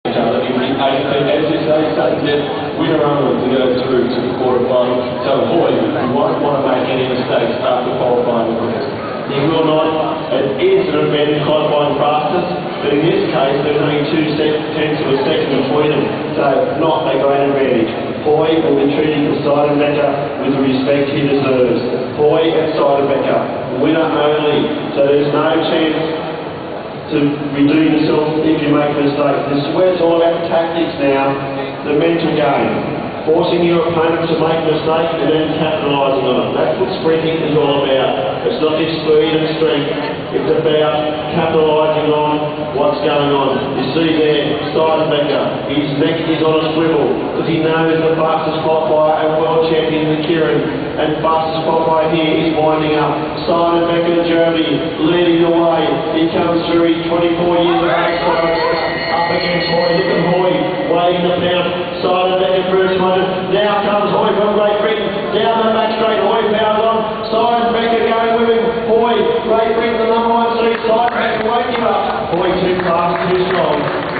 UCI Track World Cup Sprint 1/8 Finals Chris Hoy V's Seidenbe
Chris Hoy shows his form against Germany's Michael Seindenbecher at the Sydney World Cup on 01/12/07